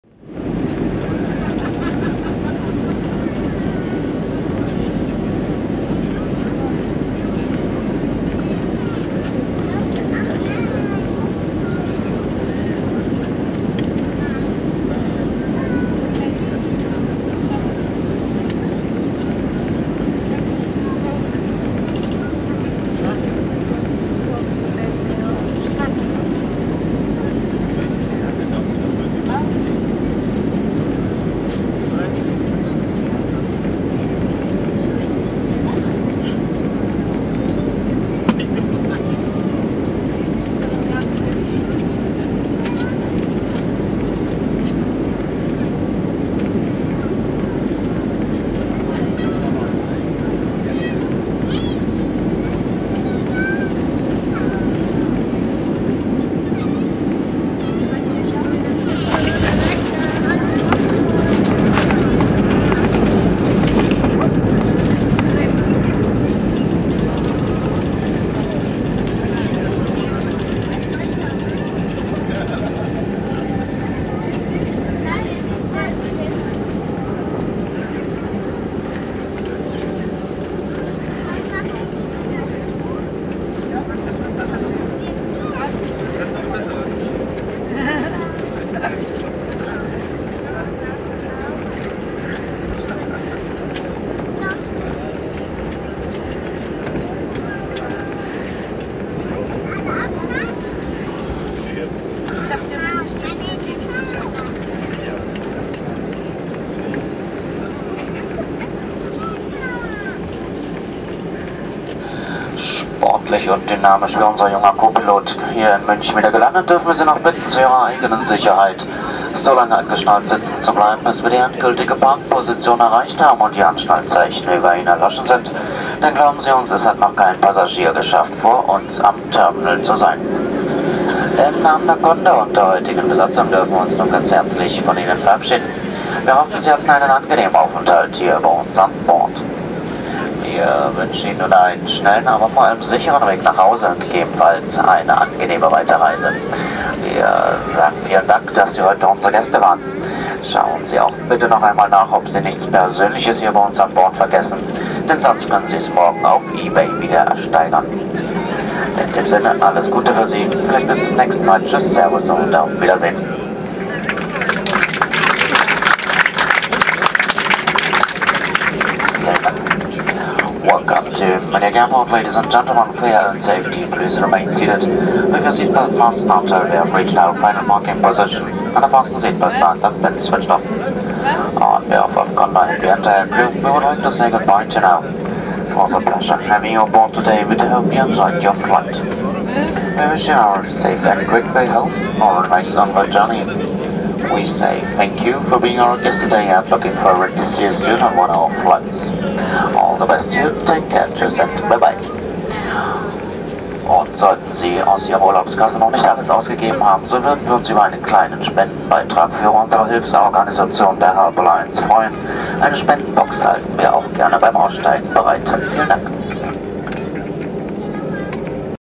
Lustige Ansage der Crew nach einer harten Landung in München
Nach einer harten Landung in MUC auf dem Flug von Lanzarote, folgte eine lustige Ansage der Crew 1:51